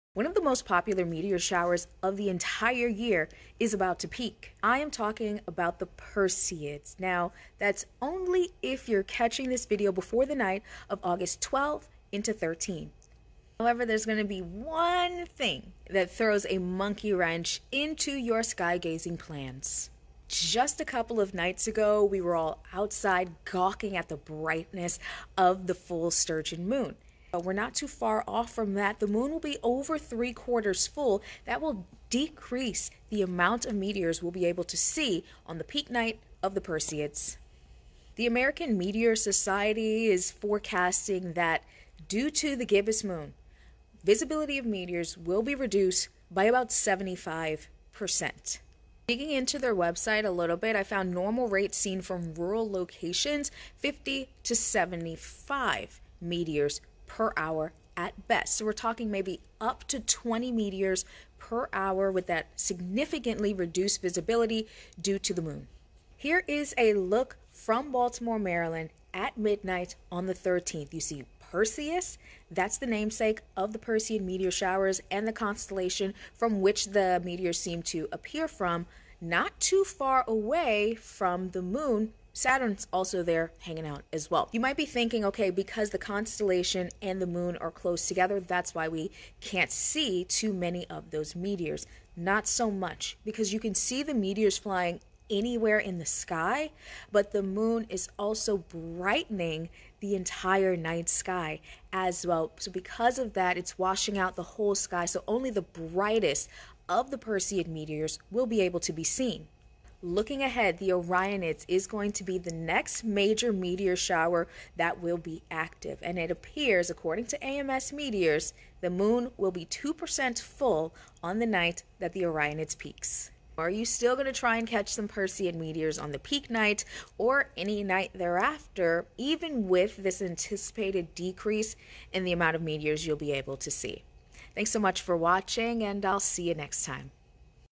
Will the Perseids meteor shower be trash this year?
Its almost time for the annually anticipated summer meteor shower - the Perseids - but it doesn't look like it'll be as spectacular this year because of something we saw just a few days before